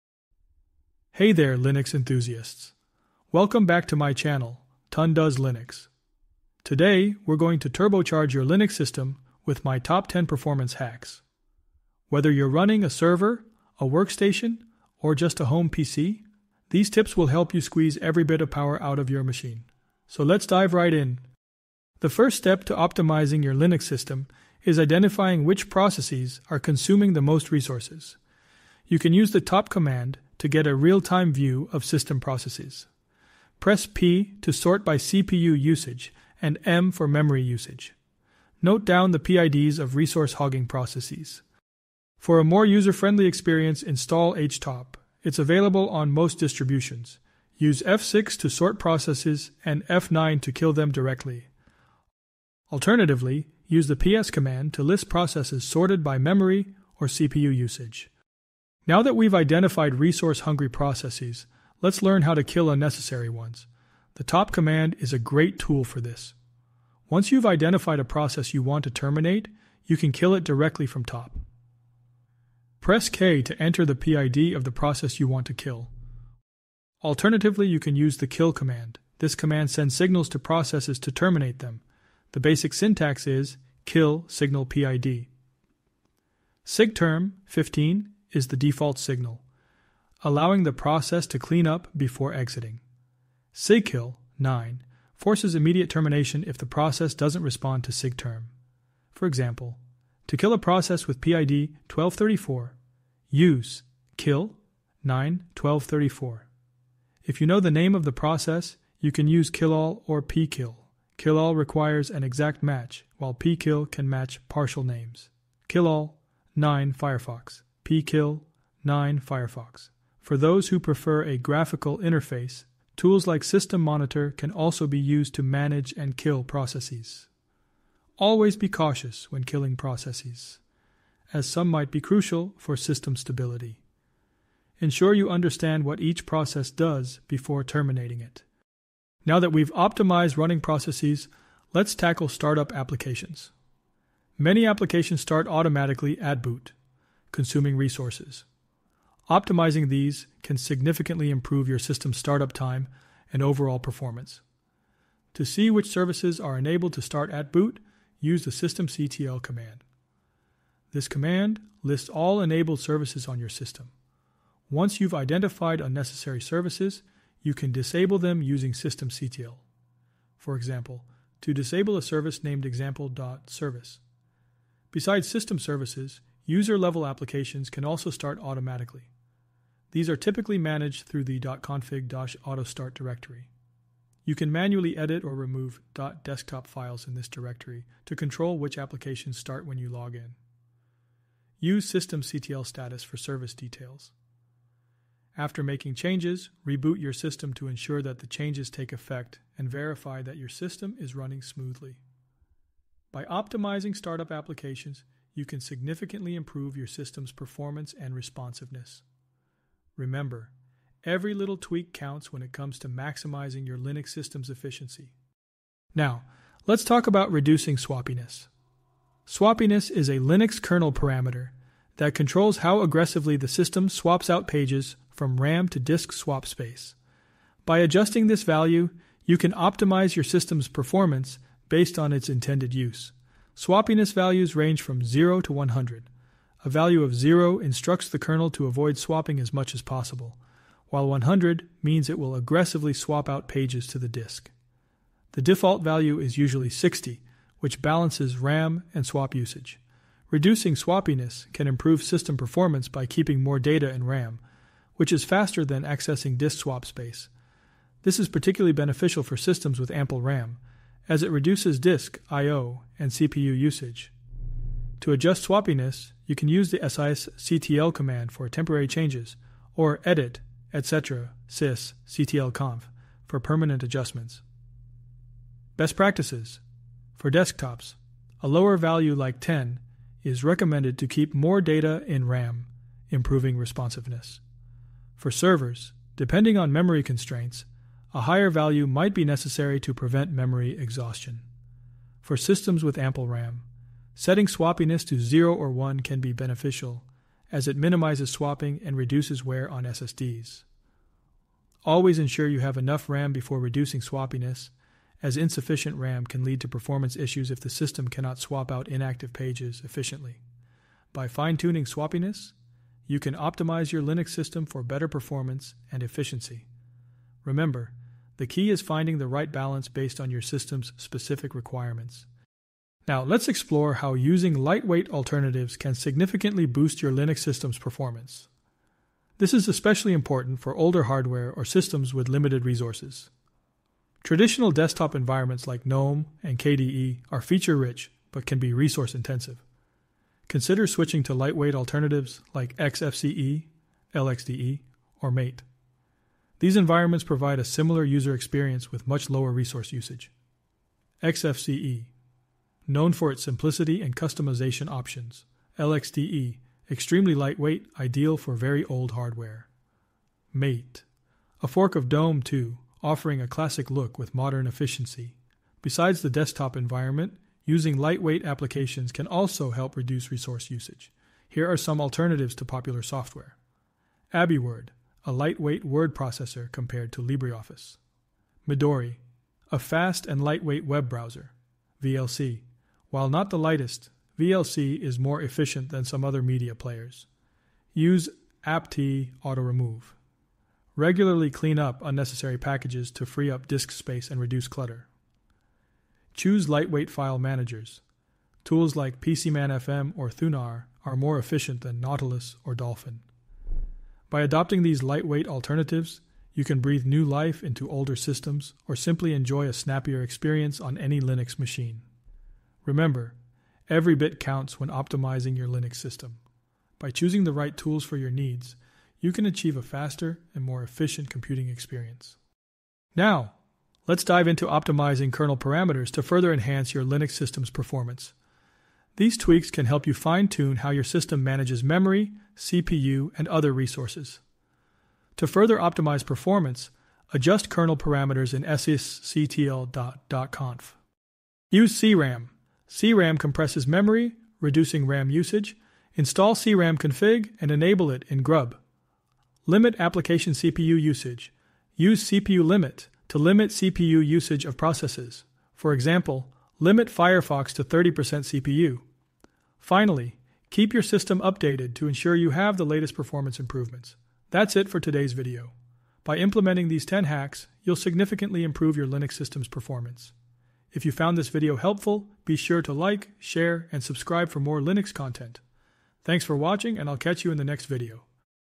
Boost Your Linux System With These Top 10 Performance Hacks
Get ready to unlock the full potential of your Linux system! In this video, I'll share my top 10 tips to reduce RAM and CPU usage, ensuring your Linux machine runs smoother than ever. From identifying resource-hogging processes to optimizing kernel parameters, we'll dive into it all. Whether you're a Linux newbie or a seasoned pro, these hacks will transform your computing experience. So, let's get started and breathe new life into your Linux system!